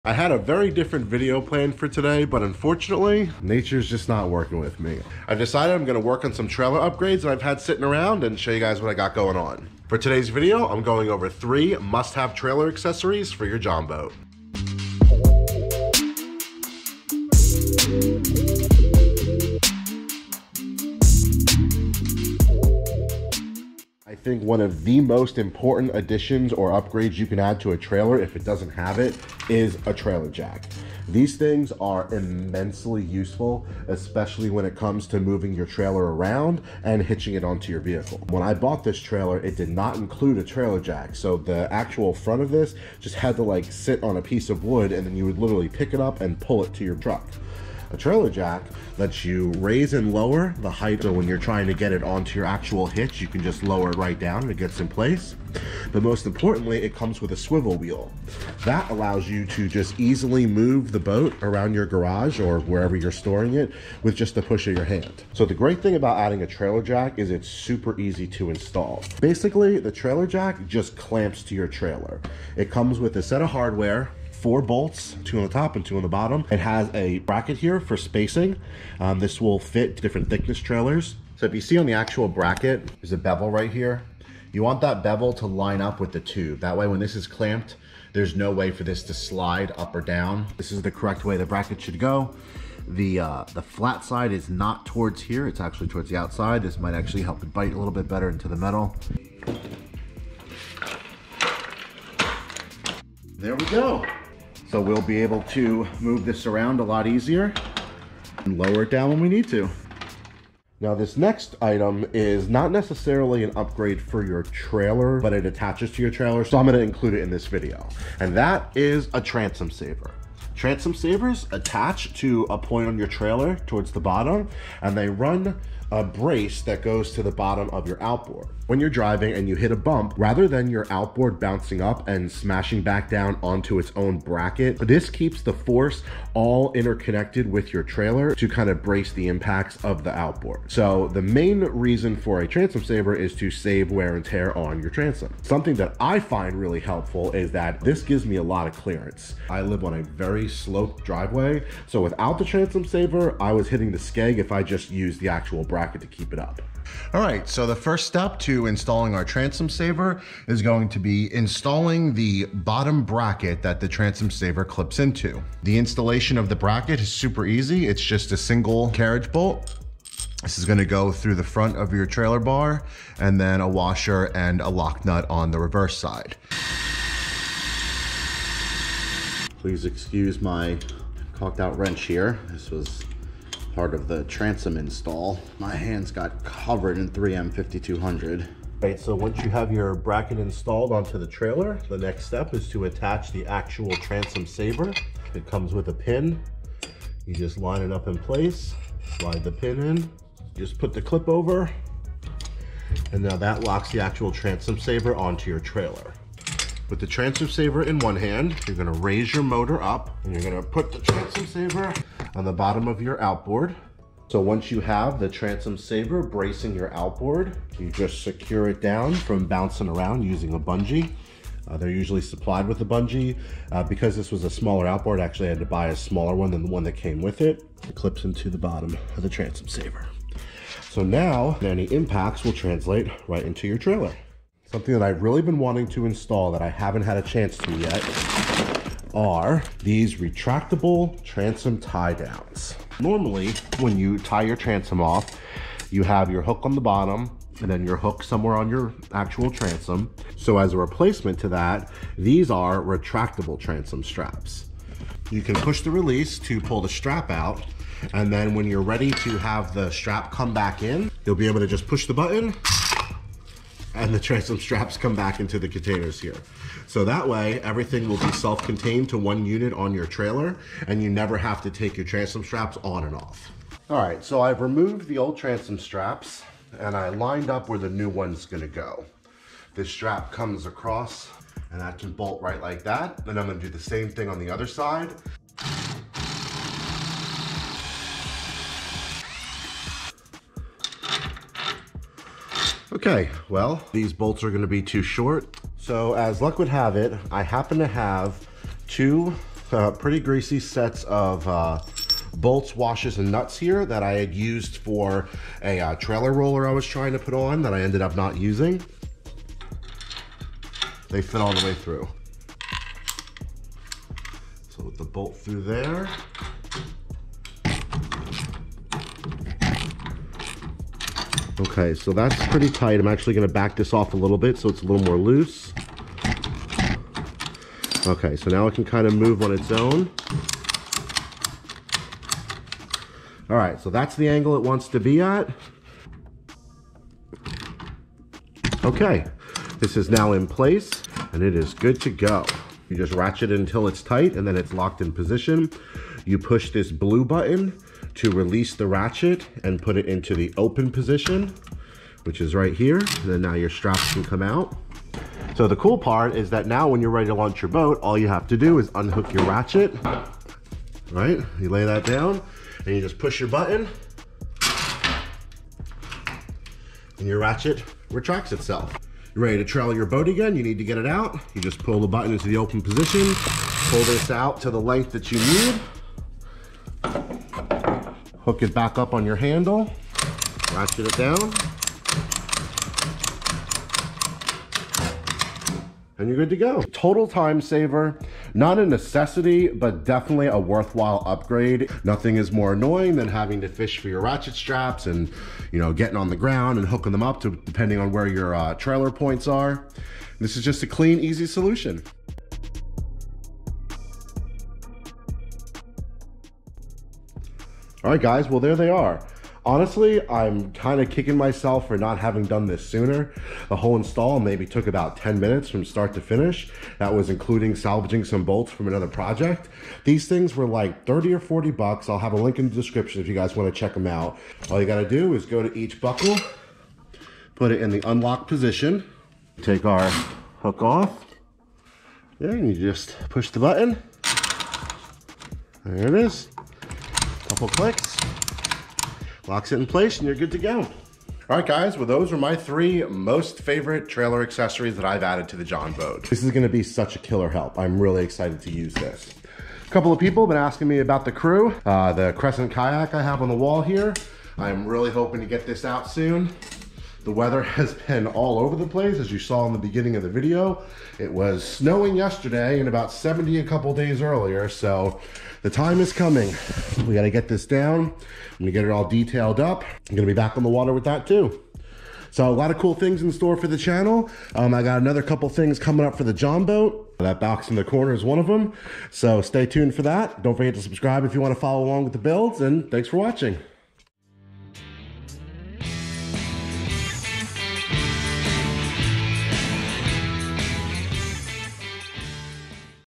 TOP 3 BOAT TRAILER ACCESSORIES & HOW TO INSTALL THEM - JON BOAT TO BASS BOAT BUILD CONVERSION
- For this video I make the best of a snowed out weekend to go over and install my top 3 trailer accessories. I hope you guys find this helpful, hit me up in the comments if you guys have any other suggestions!

–––Product Links*
#1 Retractable Transom Straps
#2 Attwood Transom Saver
#3 Swivel Trailer Jack